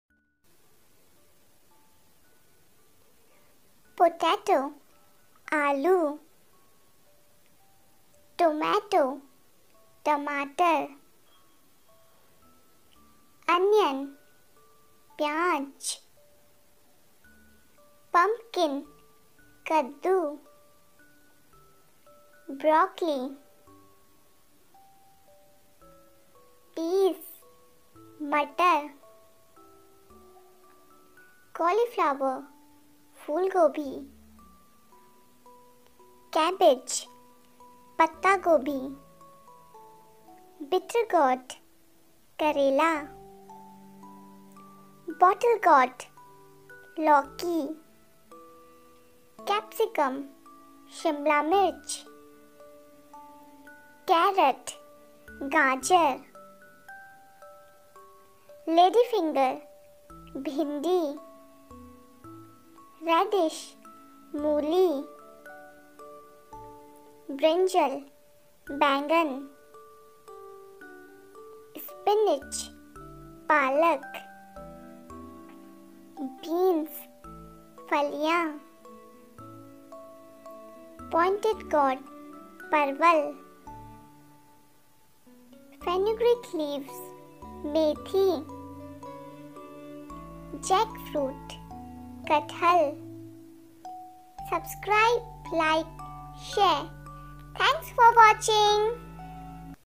Vegetables name|सब्जियों के नाम |vegetables name for kids |vegetables name in English
Vegetables names| सब्जियों के नाम | vegetables names for kids |

With the help of this video children can able to learn vegetables names.
Here 20 vegetables names with pictures.

Our some other educational videos link  are -